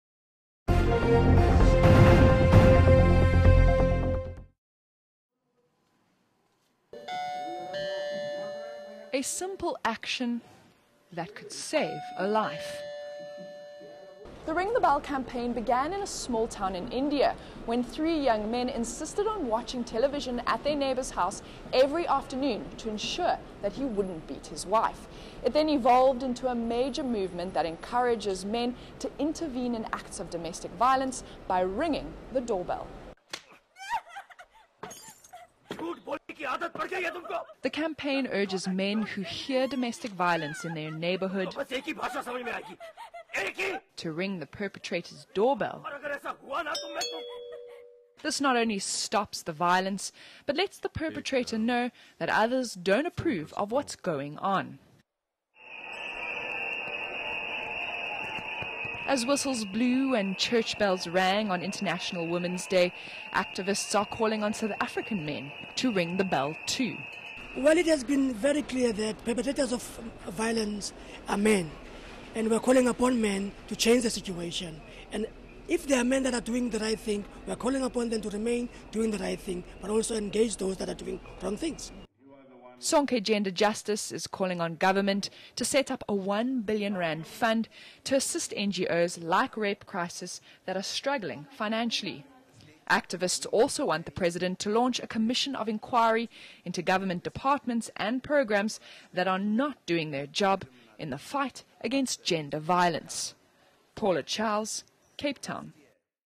Campaign calls on men to stand up against gender-based violence
Cape Town, March 9 - Bells were ringing today in memory of women who have died as result of violence.